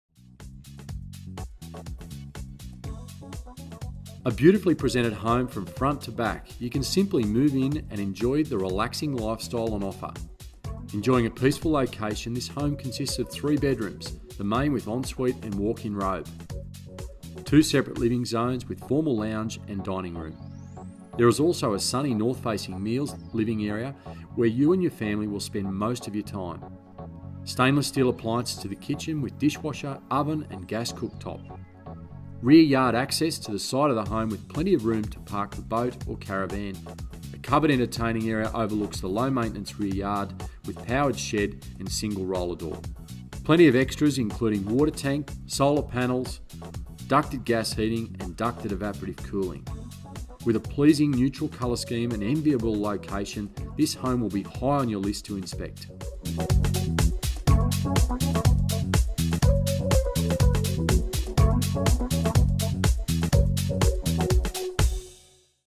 SOLD - 3 Wilton Crescent, Bourkelands, Wagga Wagga
Take a tour with Andrew Mcleod through 3 Wilton Crescent, Bourkelands, Wagga Wagga.